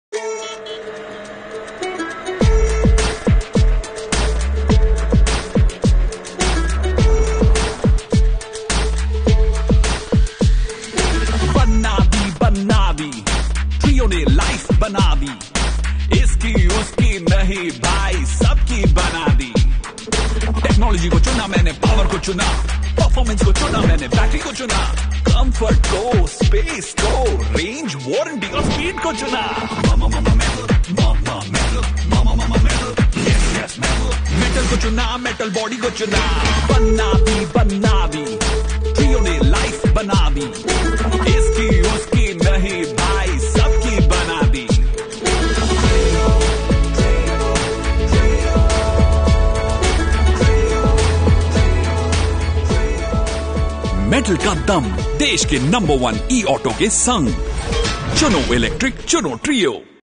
Introducing our all-new Mahindra Treo Plus, in metal body! Experience ample space,
Introducing our all-new Mahindra Treo Plus, in metal body! Experience ample space, better technology, and much more. Watch our video to discover what makes it the ultimate choice for urban mobility.
.call for Sales assistants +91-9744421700